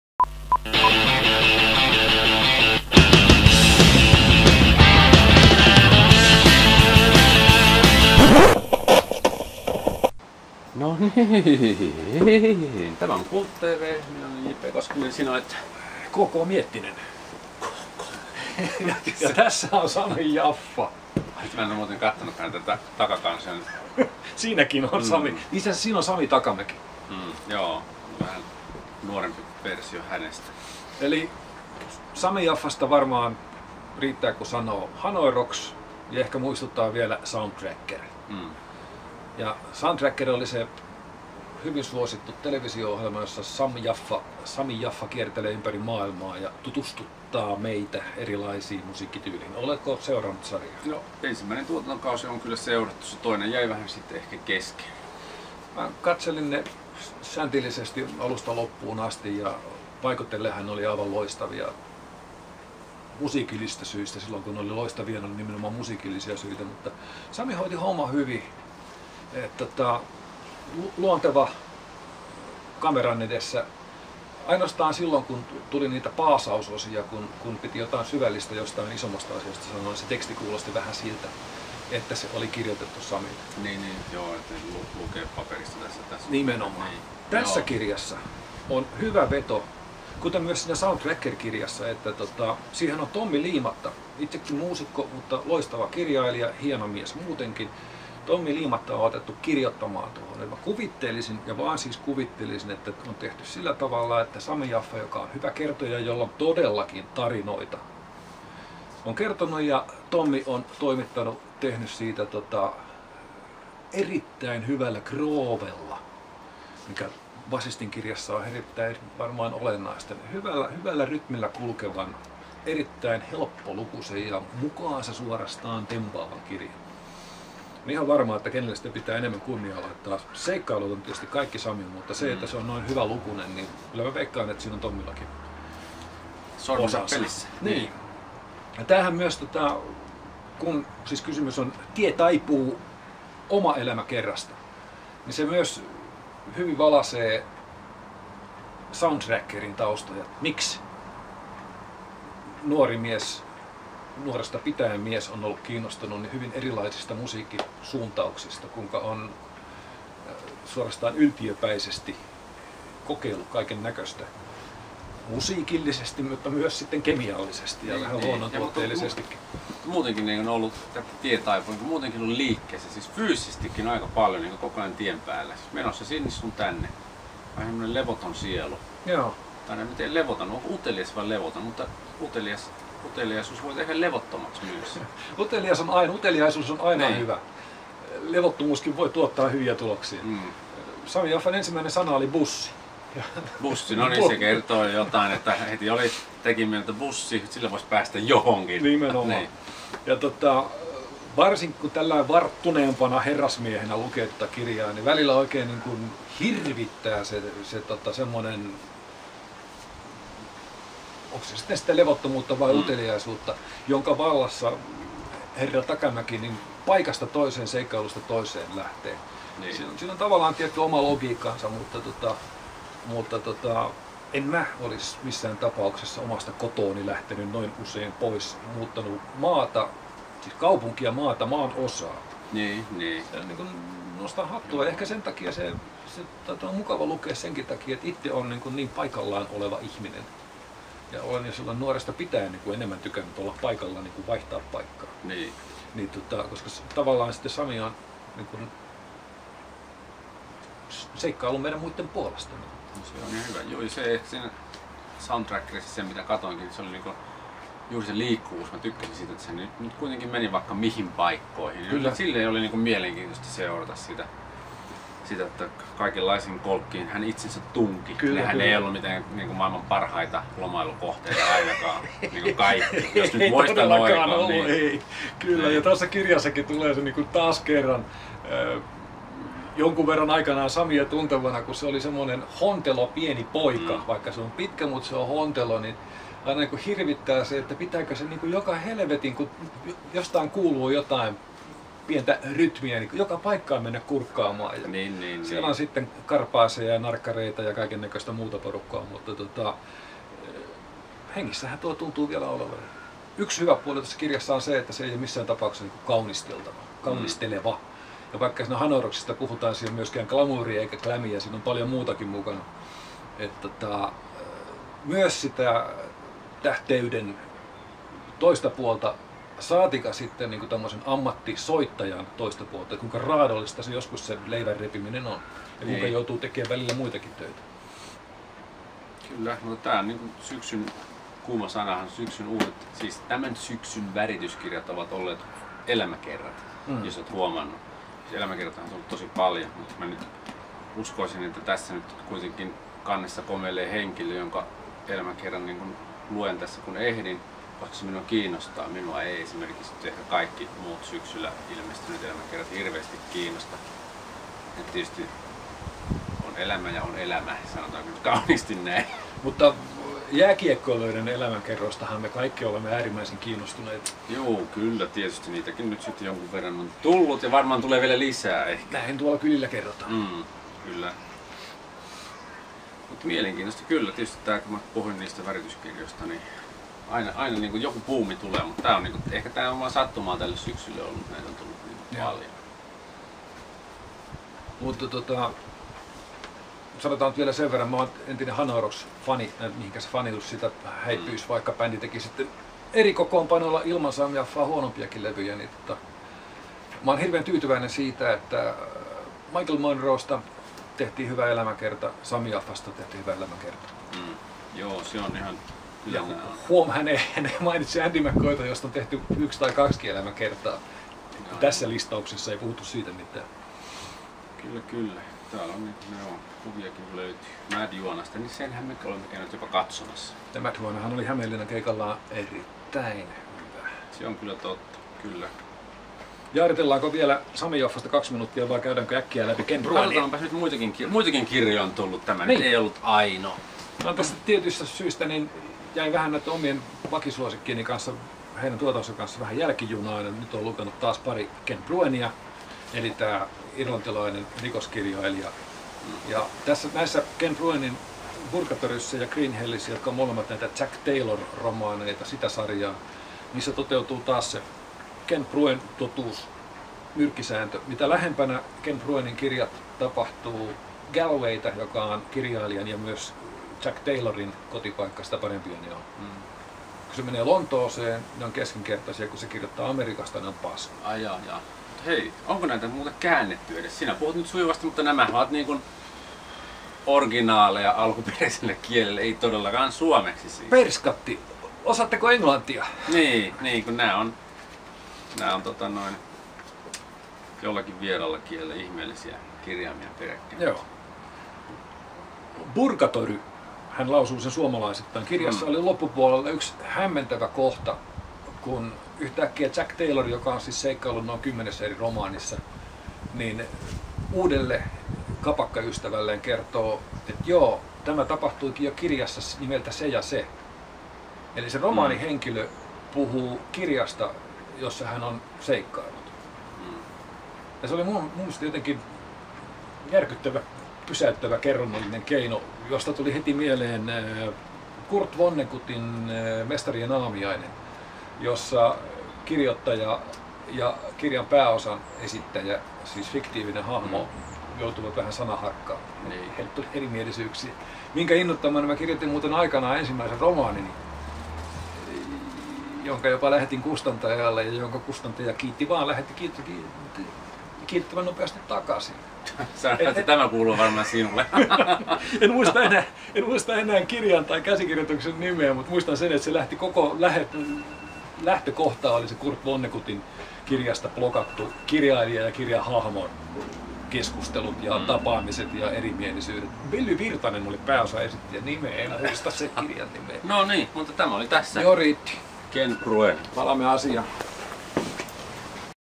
KultTV #71: Elämäkerta, Sami Yaffa!
KultTV: osa 71: Elämäkerta, Sami Yaffa!

Kimmo Miettinen ja JP Koskinen puhuvat kirjoista ja kulttuurista.
Palaute: kulttv (AT) jpkoskinen (piste) com